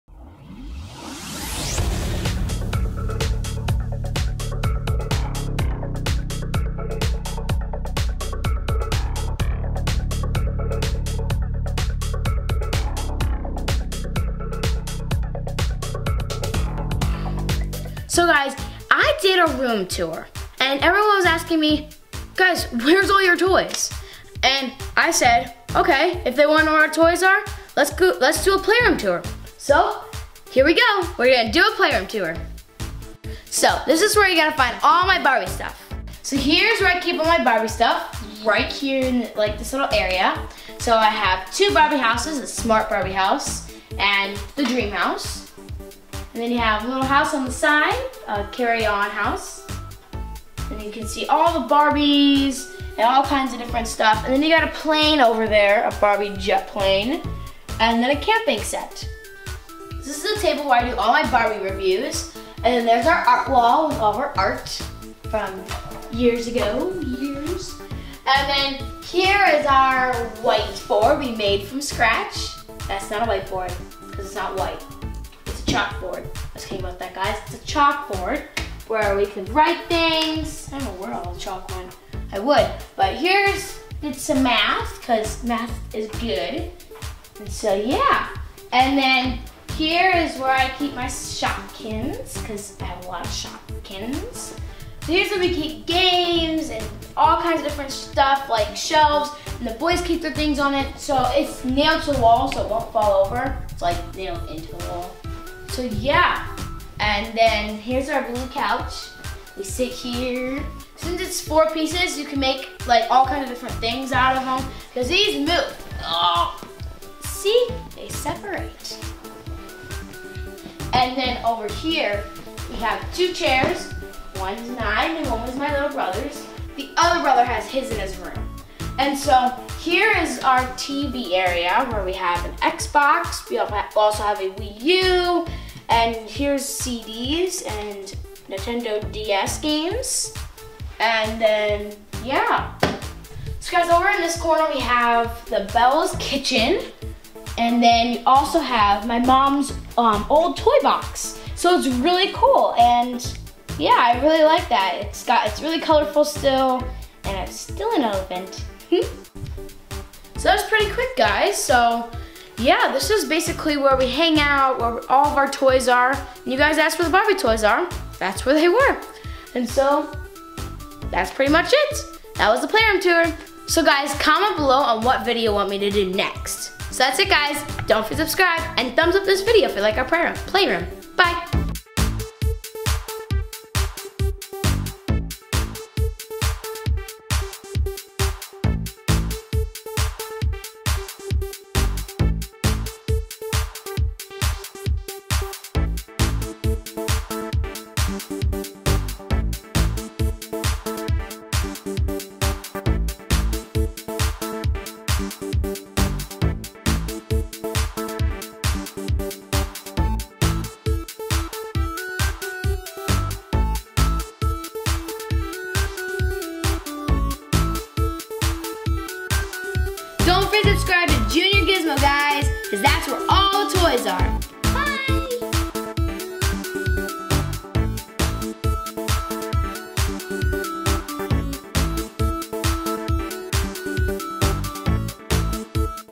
MY PLAYROOM TOUR!
My PLAYROOM TOUR!  After my "Room Tour" SO MANY of you asked where all my toys and Barbie dolls were so I thought a PLAYROOM TOUR would be a good idea!  This is the playroom that I share with my brothers. I will do a FULL HOUSE TOUR coming up soon!

FOLLOW ME ON MUSICAL.LY: @SMS_Gracie